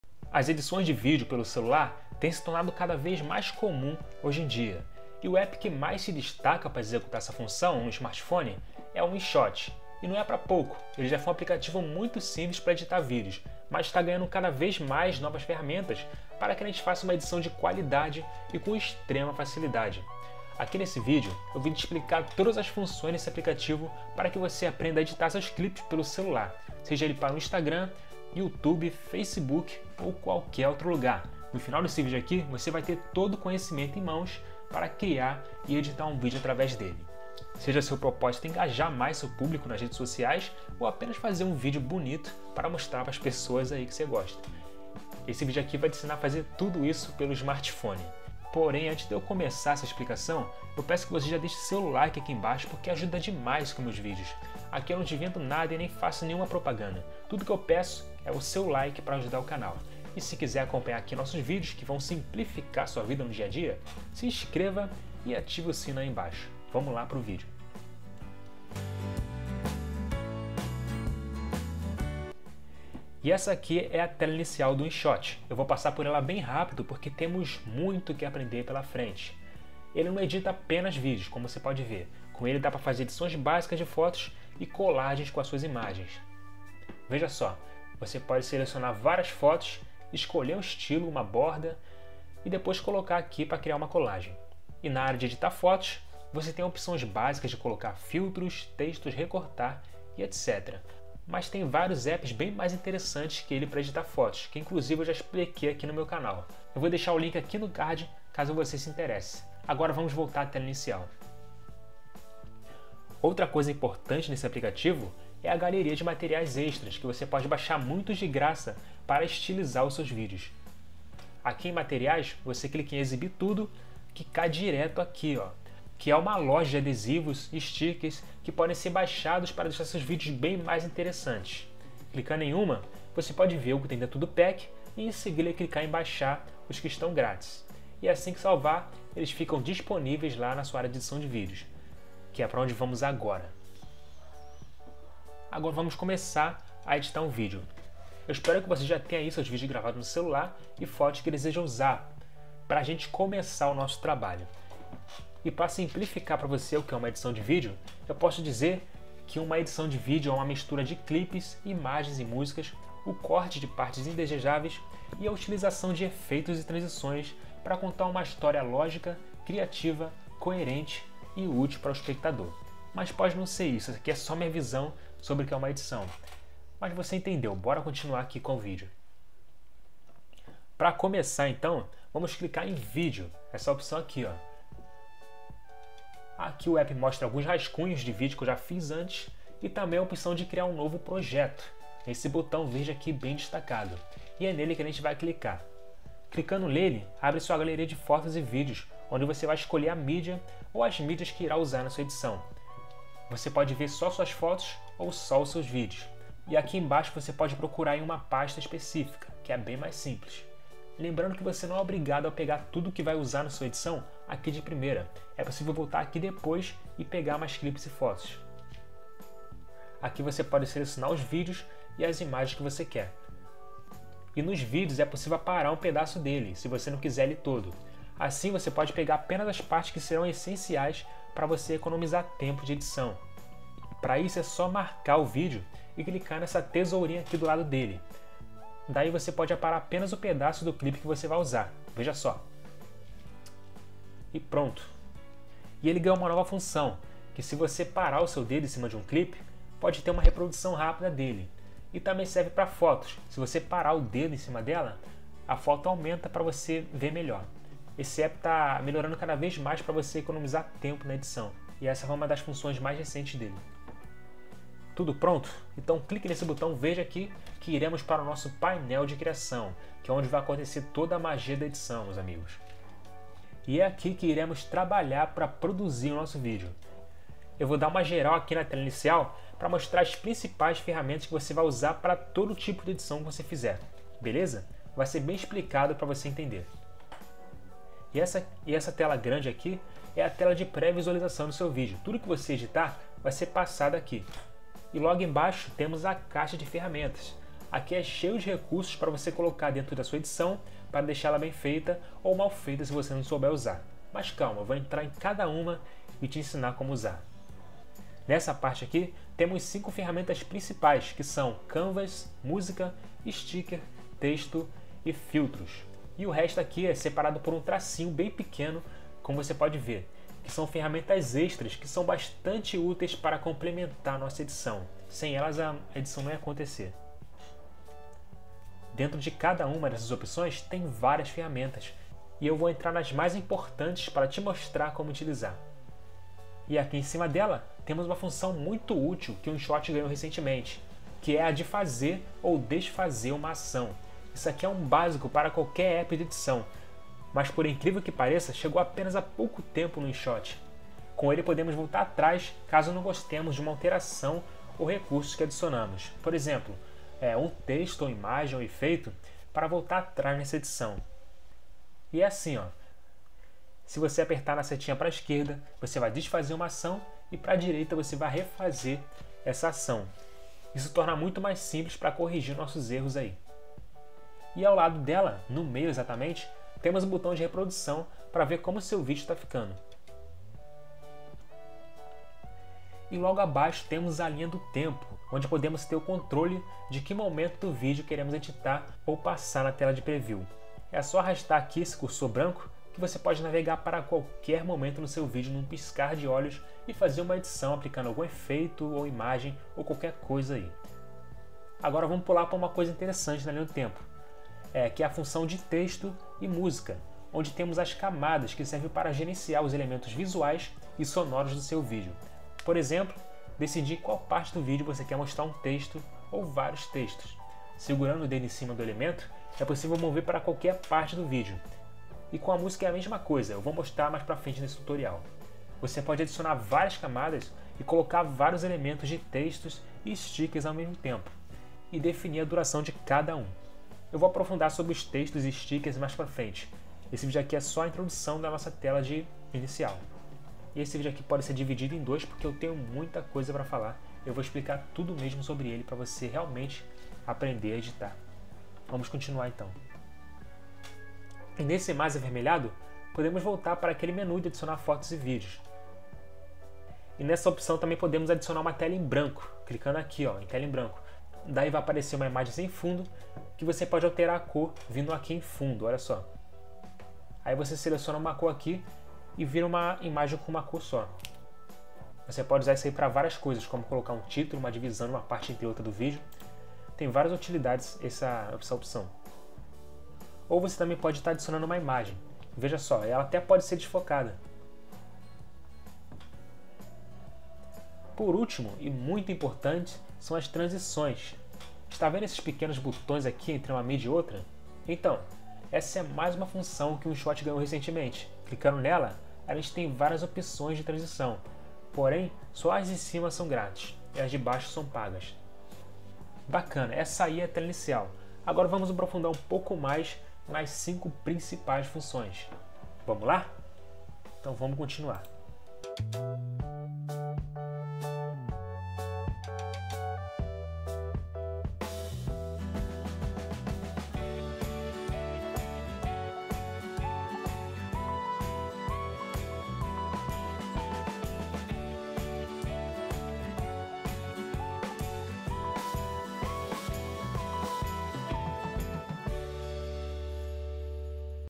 Como EDITAR VÍDEOS pelo celular usando o aplicativo INSHOT
As edições de vídeos pelo celular tem se tornado cada vez mais comum hoje em dia. E o app que mais se destaca para executar esta função no smartphone é o inshot. E não é pra pouco. Ele que já foi um aplicativo muito simples para editar vídeos, mas está ganhando cada vez mais novas ferramentas para que a gente faça uma edição de qualidade com extrema facilidade.

Aqui nesse vídeo eu vim te explicar todas as funções desse aplicativo para que você aprenda a editar seus clipes aí pelo celular. Seja ele para instagram, youtube, facebook ou qualquer outro lugar, no final desse vídeo você terá todo conhecimento em mãos para criar e editar um vídeo através dele.

Se seja seu propósito engajar mais mais seu público nas redes sociais ou apenas fazer um belo vídeo para mostrar as pessoas, esse vídeo aqui te ensinará como fazer isso pelo seu smartphone.

O que você irá aprender?

1. Como pegar os videos e fotos para editar
2. Como usar a linha do tempo e as camadas
3. Marerias extras